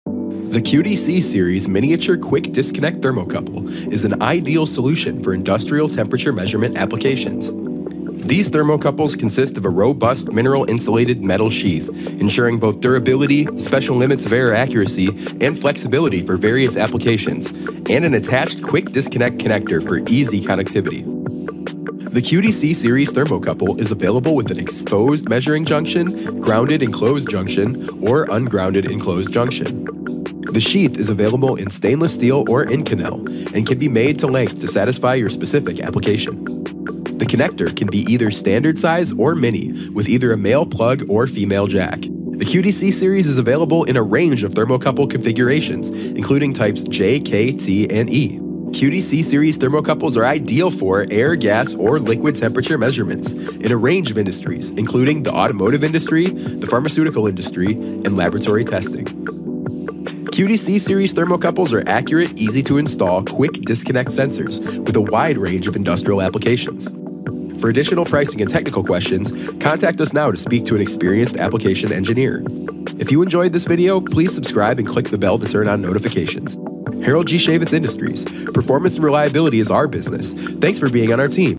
QDC Series Quick-Disconnect Thermocouple Sensor Overview
The QDC miniature thermocouples are an ideal solution for surface temperature measuring applications. These thermocouples feature an attached quick disconnect connector for easy connectivity and special Limits of Error accuracy. The mineral sheath is insulated for high strength and bendable. Ideal for Air, Gas, or Liquid temperature measurements